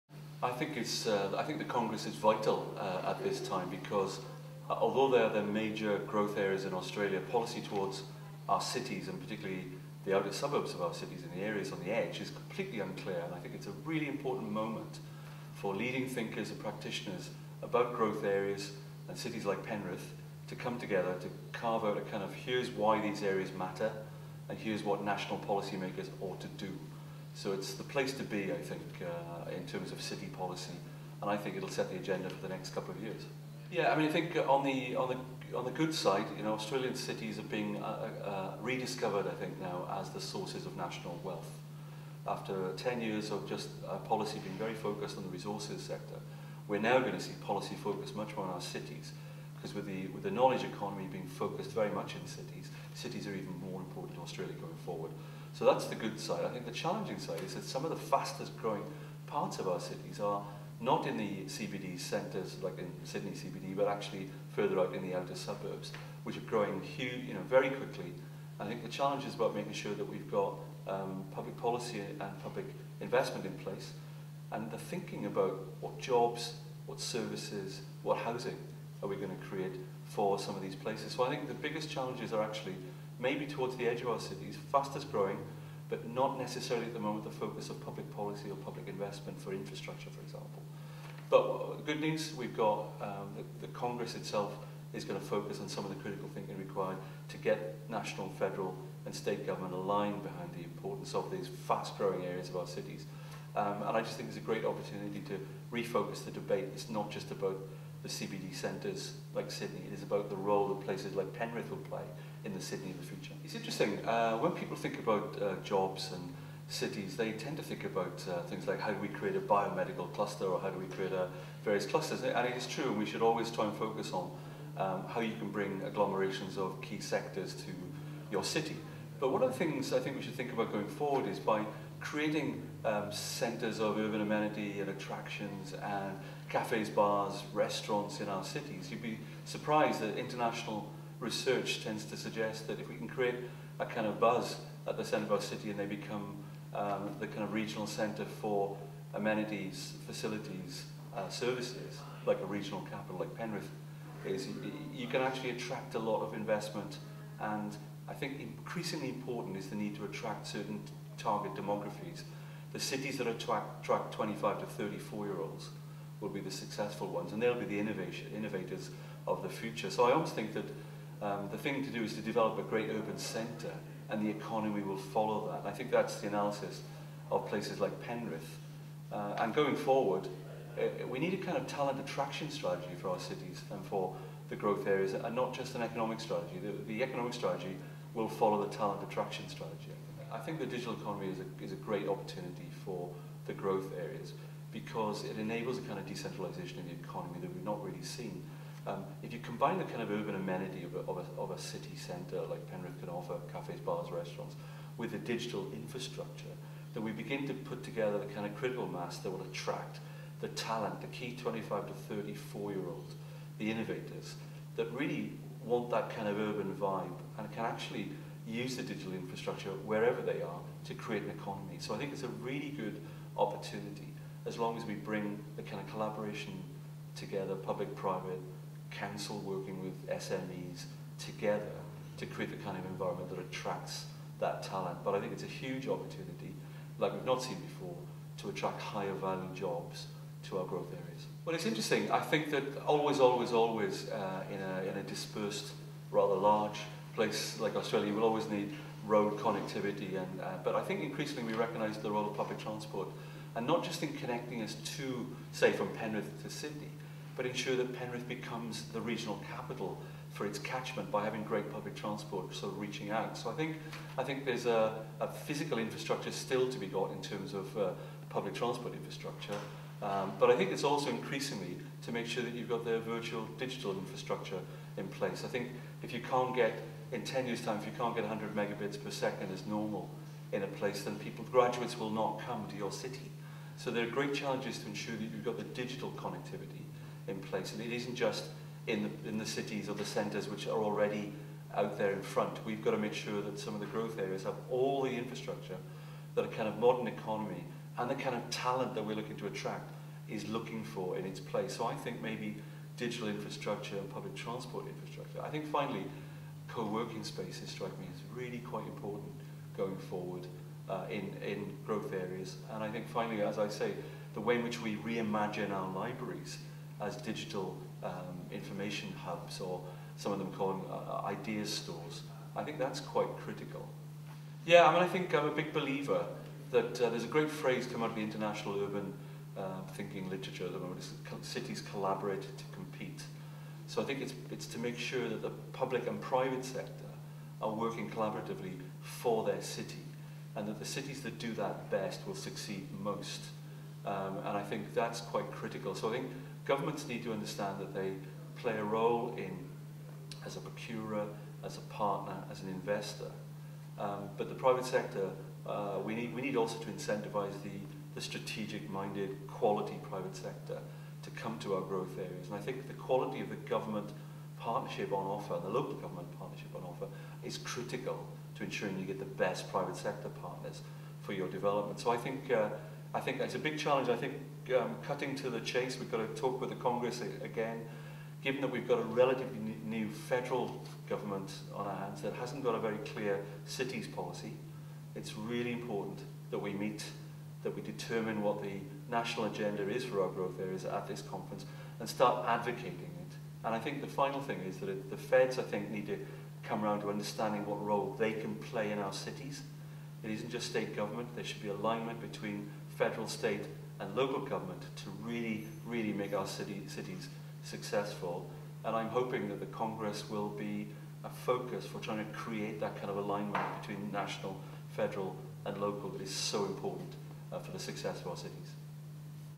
Keynote Speaker for the 2014 NGAA Congress, Dr Tim Williams
Dr Tim Williams talks about his views on growth in the lead up to the Congress.